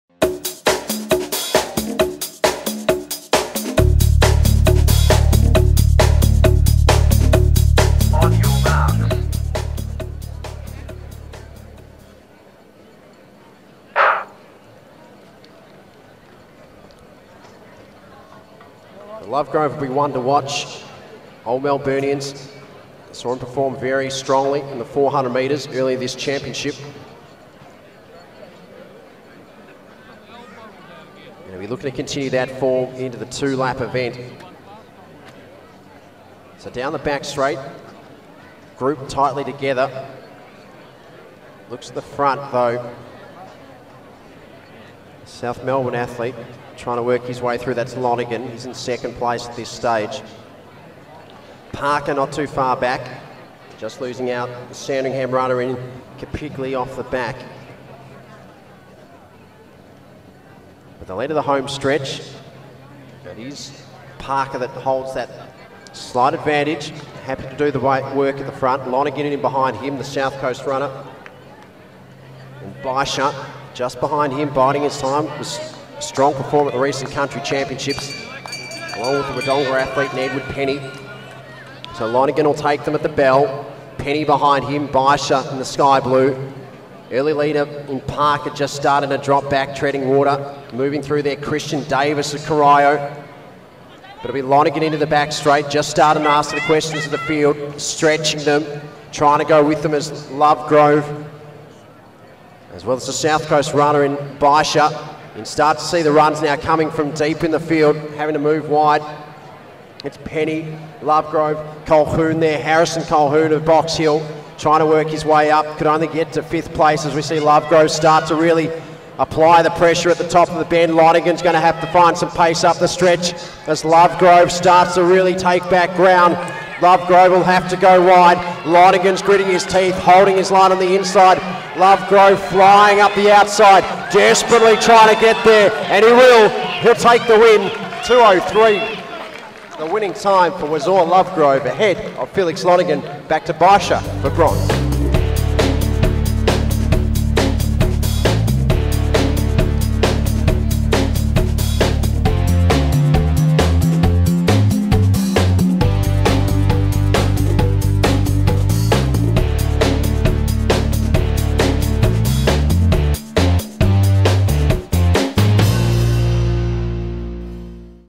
2014 Vic Jnr Champs. Men U16 800m.
2014 Victorian Junior Track & Field Championships.
Held 14-16 February at Lakeside Stadium, Melbourne.
Women Under 16 800m Final.